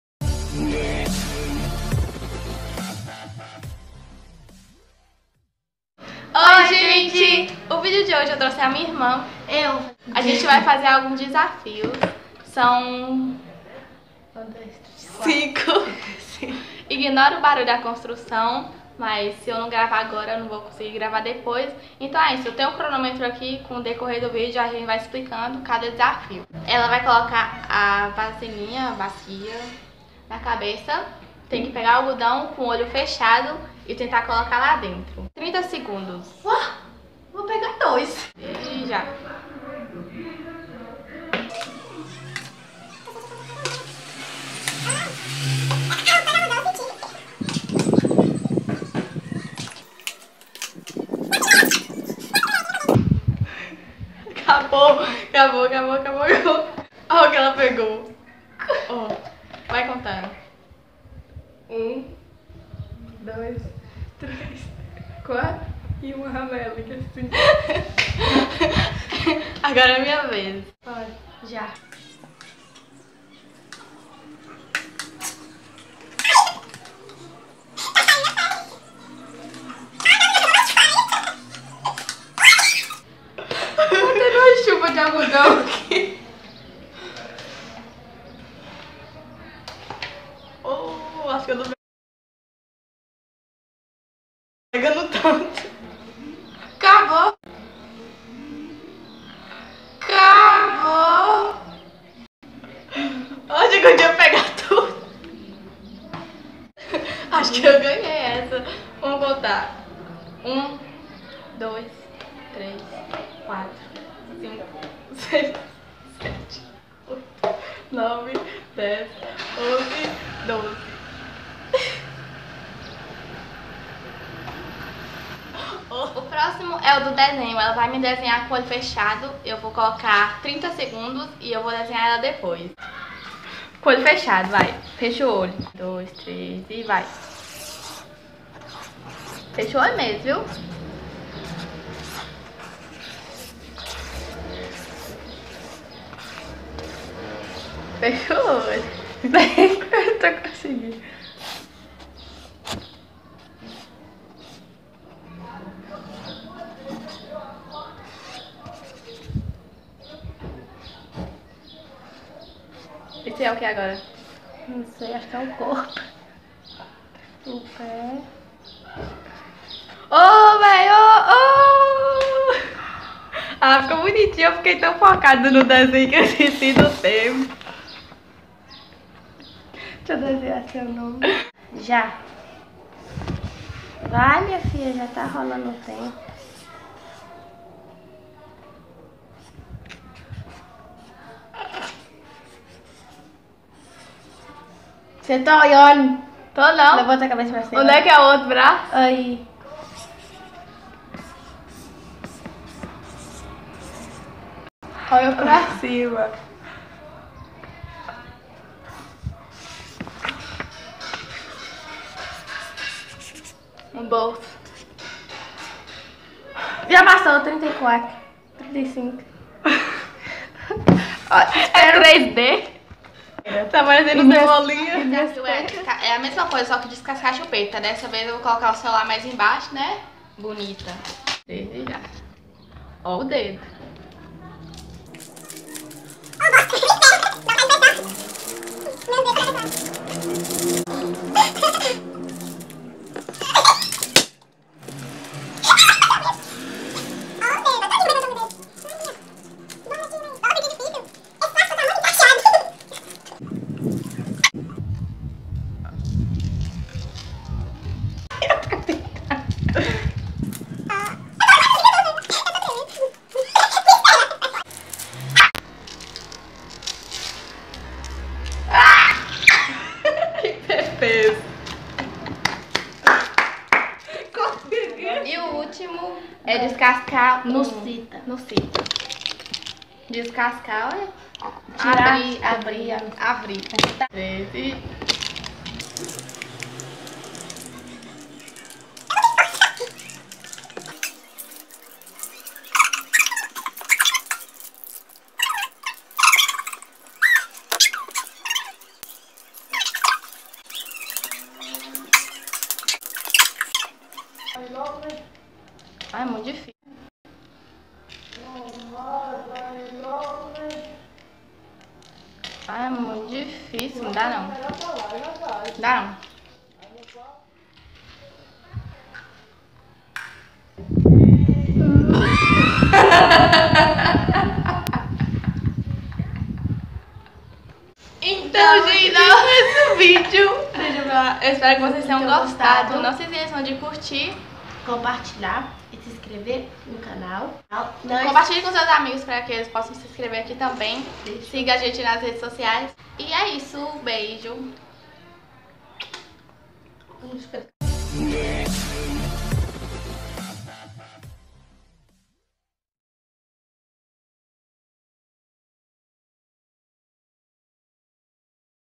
Irmã X Irmã 😲❤
Ooii seus lindos , tudo bem ???
No vídeo de hoje, eu gravei alguns desafios com minha irmã, ta bem engraçado .

➡ Deixem sugestões de vídeos mores 😍

Um beijo, fiquem com Deus 😘😘